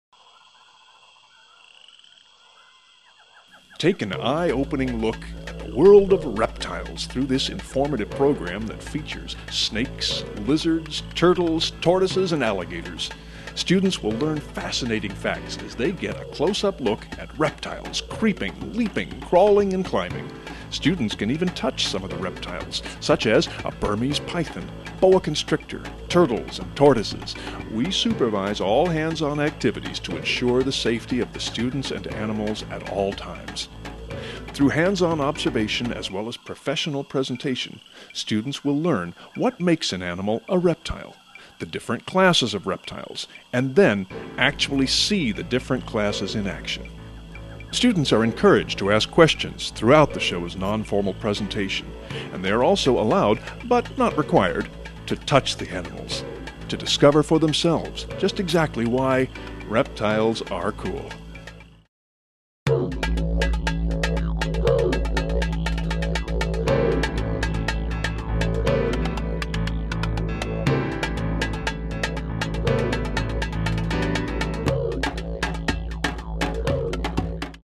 School Assembly Programs - Reptiles Are Cool
School Assemblies - Reptiles Are Cool! introduces students to the world of Reptiles...snakes, lizards, turtles, alligators